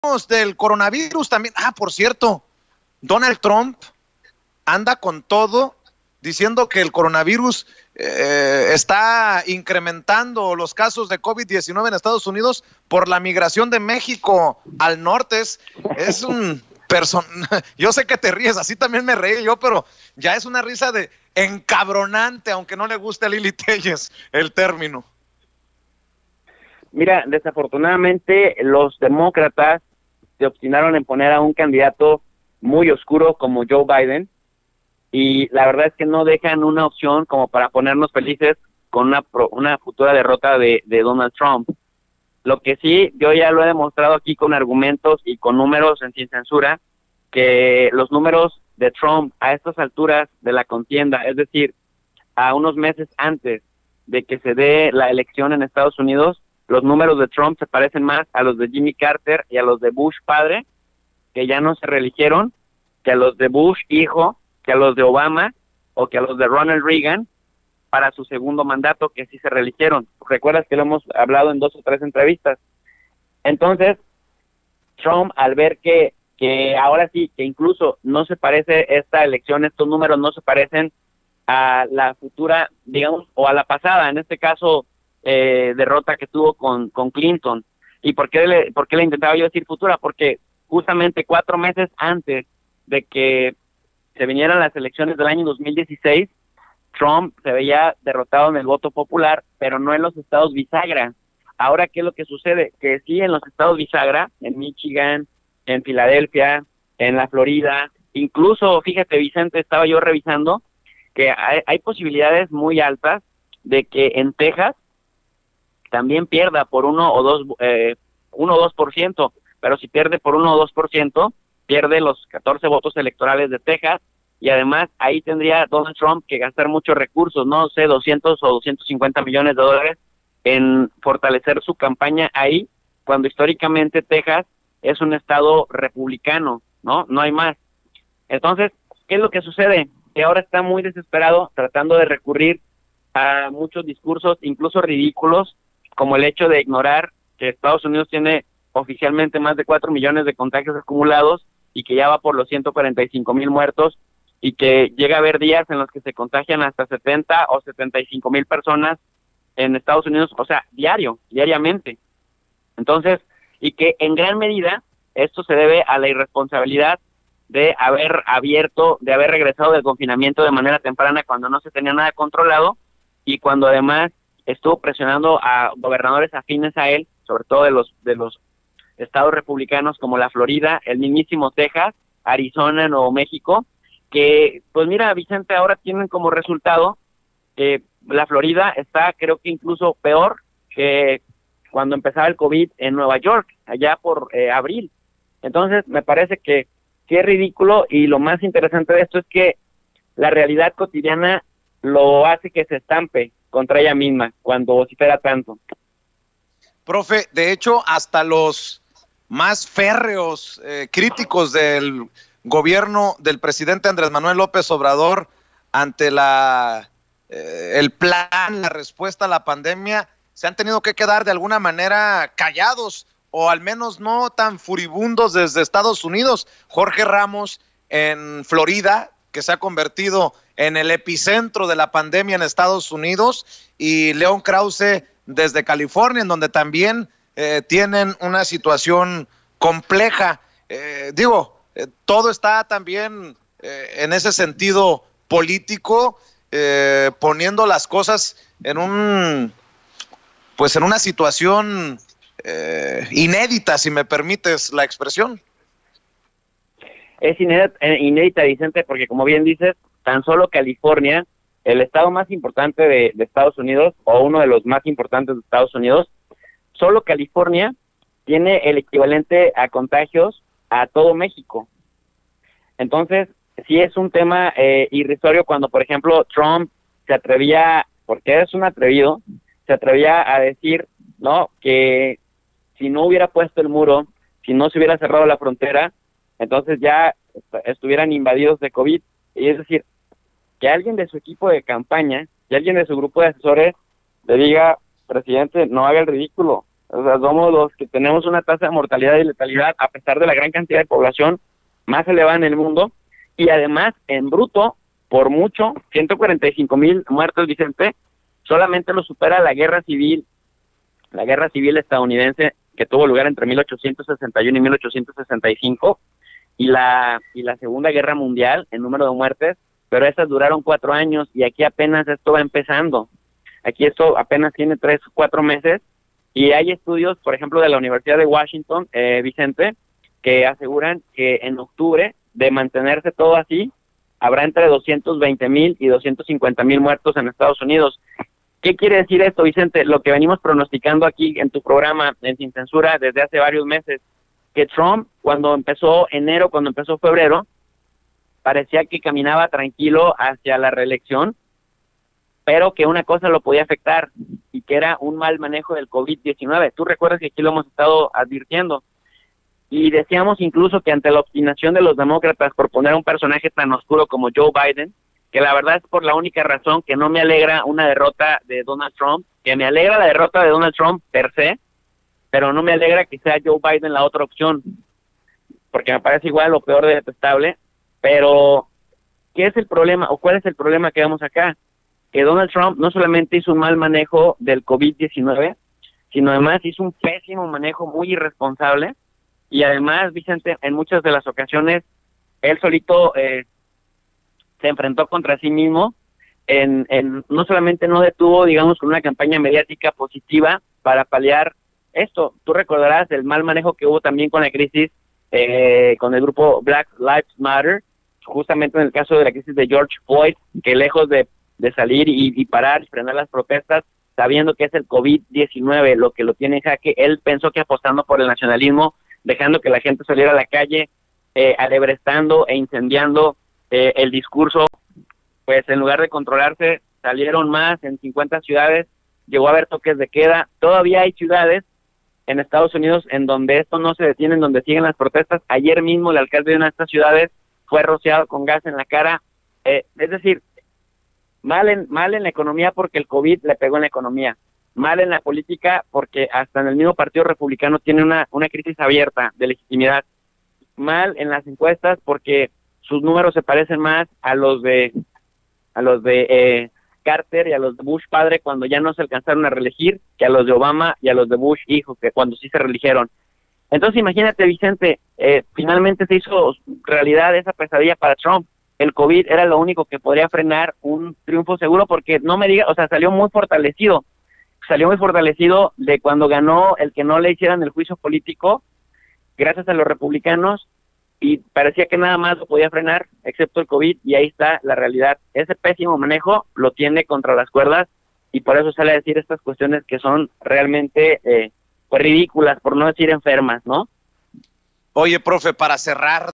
LÓPEZALMEJO: DESAFORTUNADAMENTE NO HAY OPCIÓN A TRUMP EN ELECCIONES
JesúsLópezAlmejo TuProfeDeRI en YouTube y Facebook/ Twitter: @Jjlopez_almejo nos dijo que desafortunadamente los demócratas se empeñaron a poner un rival muy oscuro para Trump, Joe Biden